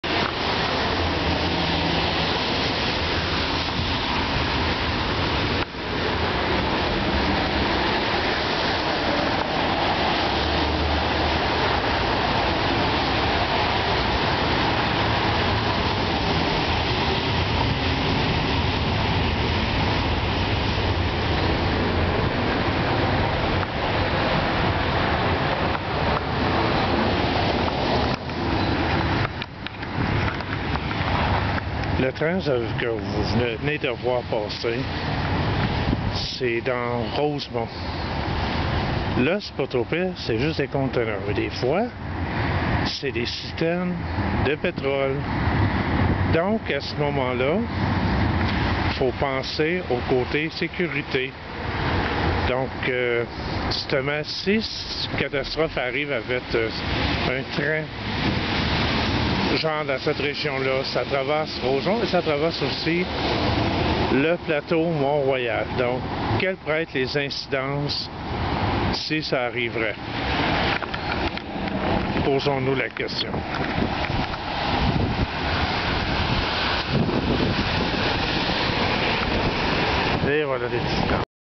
Les trains dans Rosemont
Voilà j'attendais l'autobus coin Masson et De Lorimier et ca faisait déjà 5 minutes que ce train passait sur le viaduc et alors je me suis souvenu de Mégantic, et je me suis demandé ce qui arriverait si une telle catastrophe arrivait ici ?

N'oublions pas que Rosemont-La Petite-Patrie est l'un des arrondissements des plus peuplé

De plus là moi où j'y vois un plus grand danger c'est que cette voie ferrée traverse pas seulement Rosemont-La Petite Patrie mais aussi le Plateau Mont-Royal et Hochelaga-Maissonneuve aussi je crois .

P.S: Vous excuserez disons mon erreur technique lorsque j'ai voulu parlé à la caméra j'avais l'appareil trop prêt de mon visage.